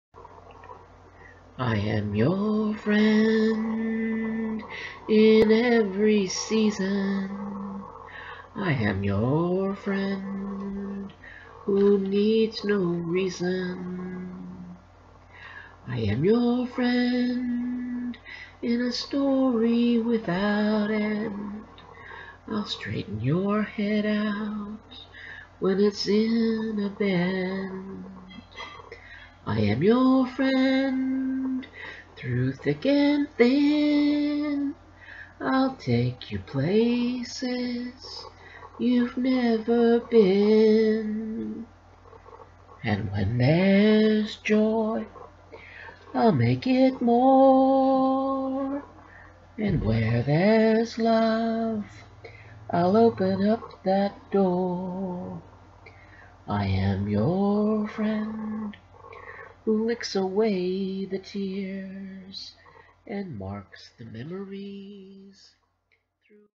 Ad Dogs Unleashed Present, "Nothing Sells Like Happiness."
Ad Dogs Unleashed is an advertising agency by Heidi Hansen that uses dog creativity in art and writing and joy to sell services and products. Copyright2018HeidiDhansen.